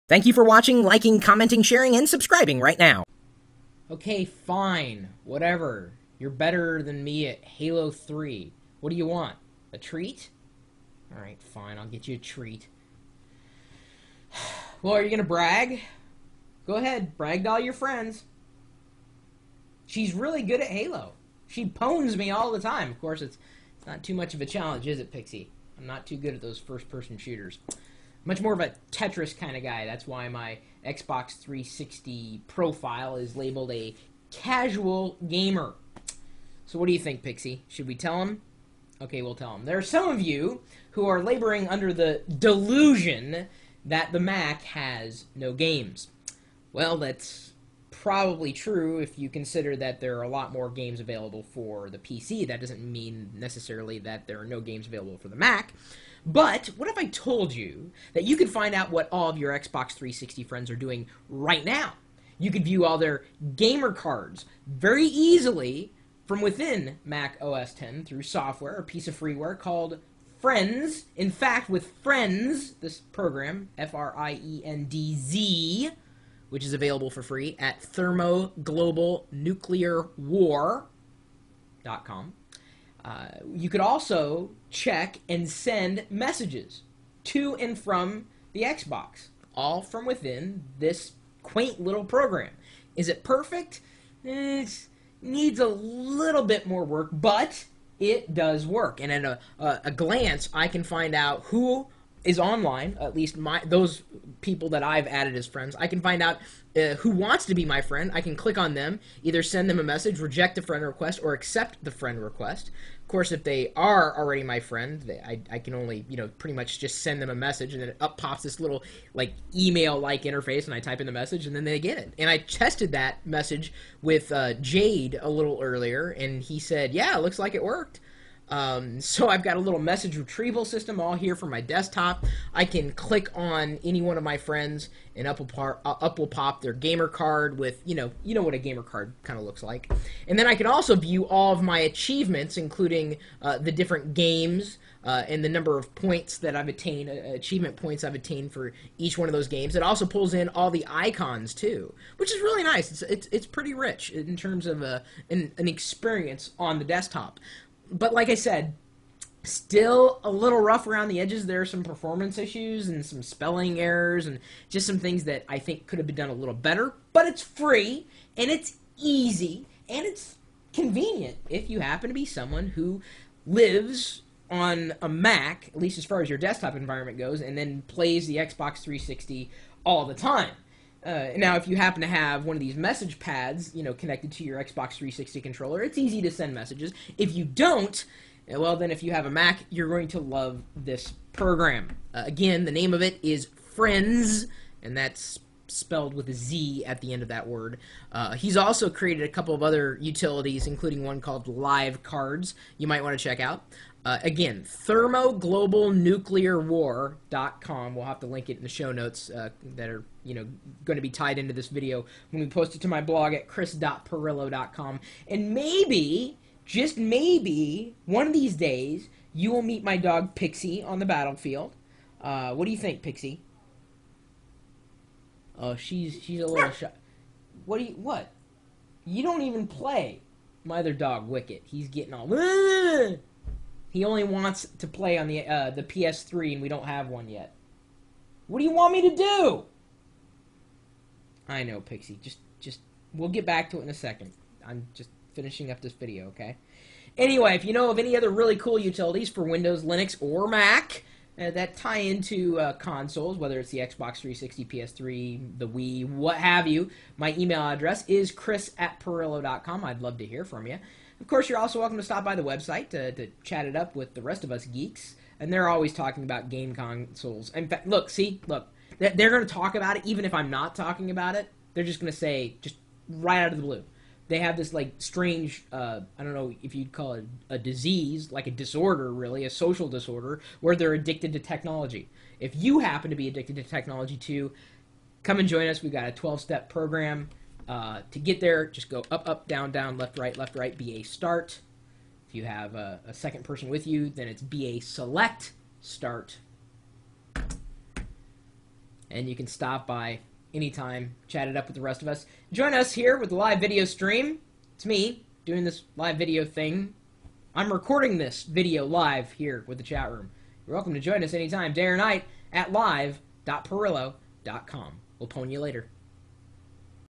What are your Xbox Friends Usually Doing?
- There are many of you who believe that there are no games for the Mac. That's probably true if you consider there are a lot more available for the PC. What if I told you that you can find out what all of your Xbox 360 friends are doing right now? You can view all of their gamer cards very easily from within OS X. Blog post /2008/06/25/what-are-your-xbox-friends-usually-doing/